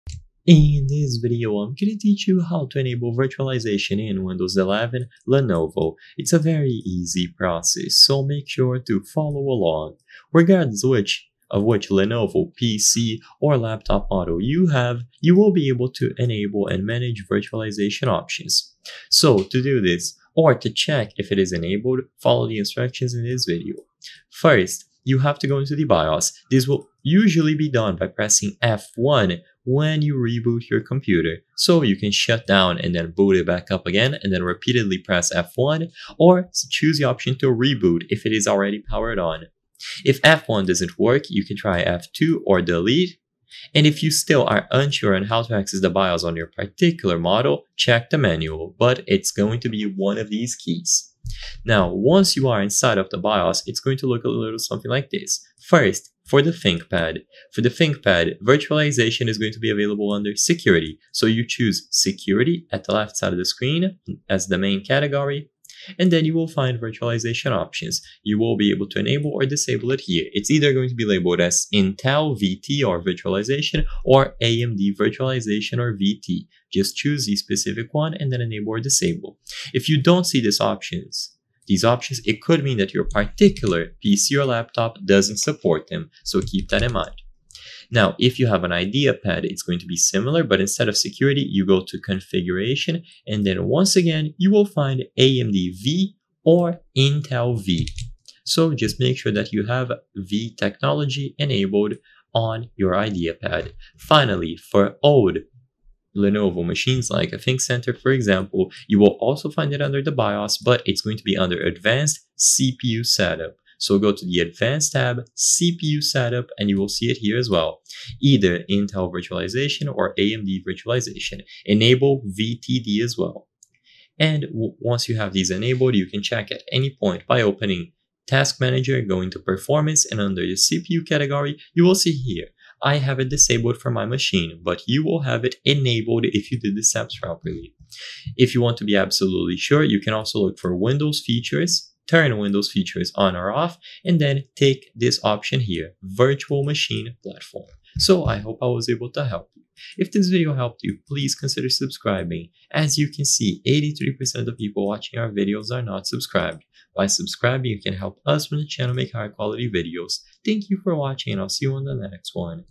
How To Enable Virtualization in Windows 11 Lenovo (2025) - Step by Step
In this video I will clear your doubts about how to enable virtualization in windows 11 lenovo, and whether or not it is possible to do this.